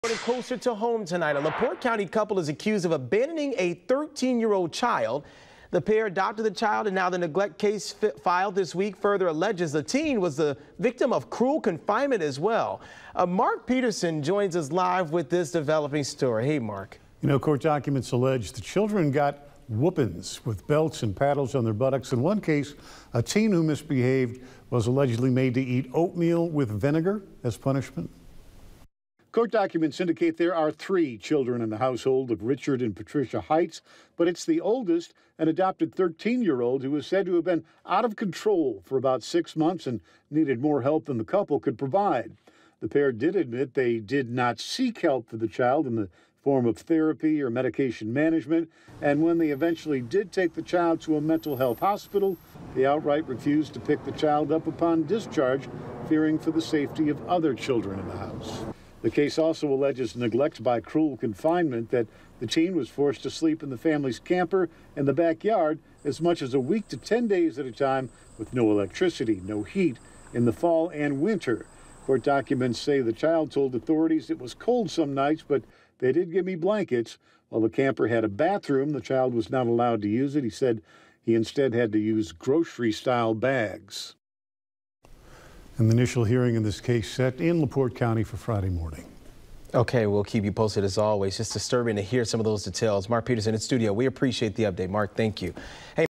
LaPorte County couple accused of abandoning adopted teen
The neglect case filed this week further alleges that the teen was the victim of cruel confinement.